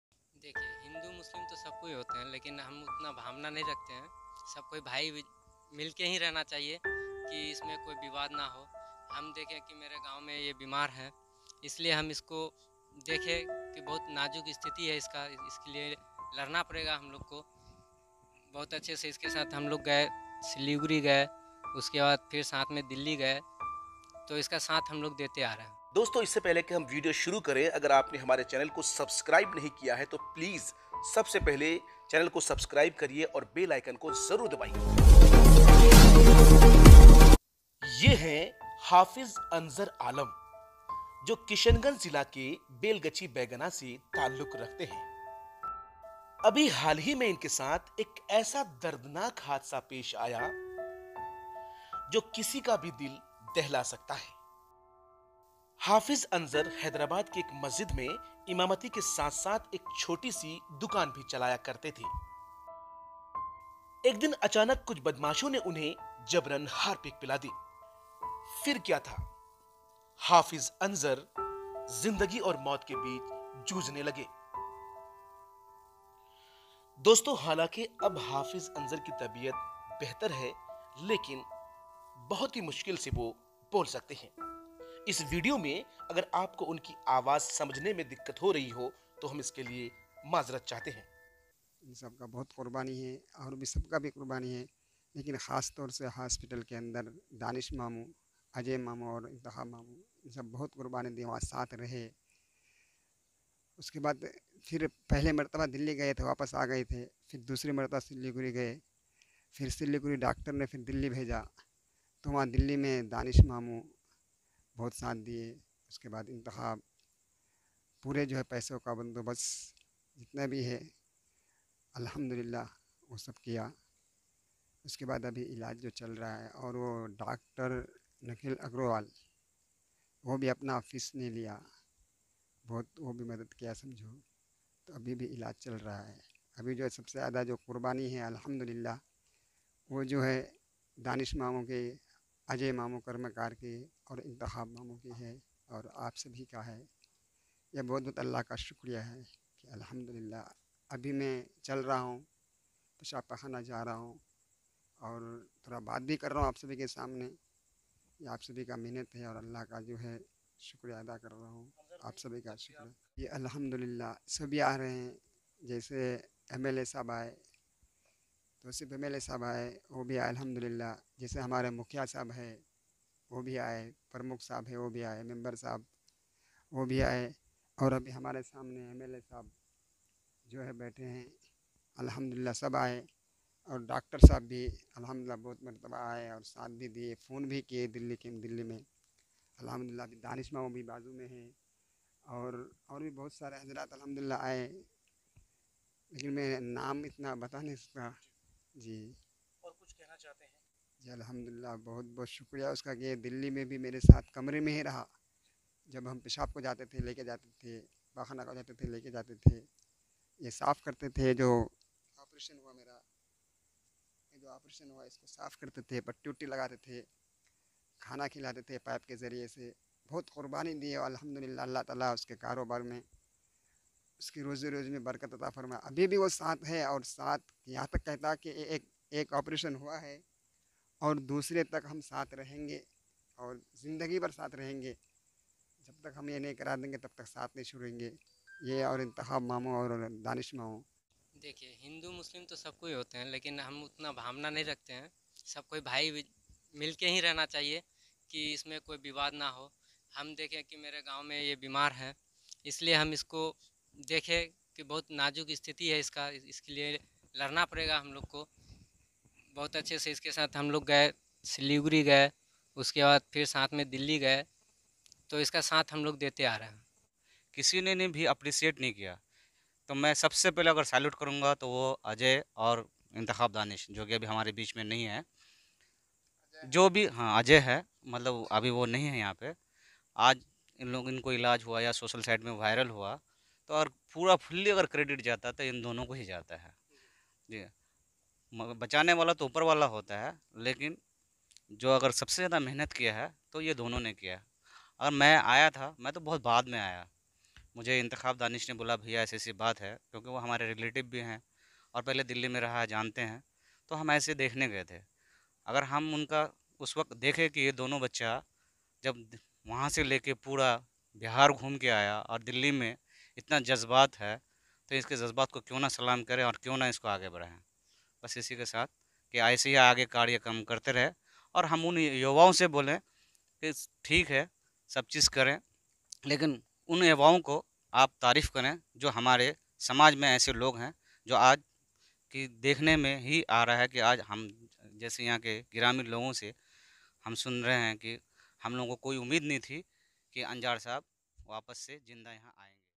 Hydrabad में मस्जिद के इमाम को पिलाया गया हार्पिक । मदद के लिए बिहार से आए हिन्दू भाई।
Kishanganj ke Hafiz Anzer Alam ko Hydrabad me pilaya gaya Harpic. Madad ke liye Aage Aye Hindu Bhai. Behad Emotional & Heart Touching Video.👇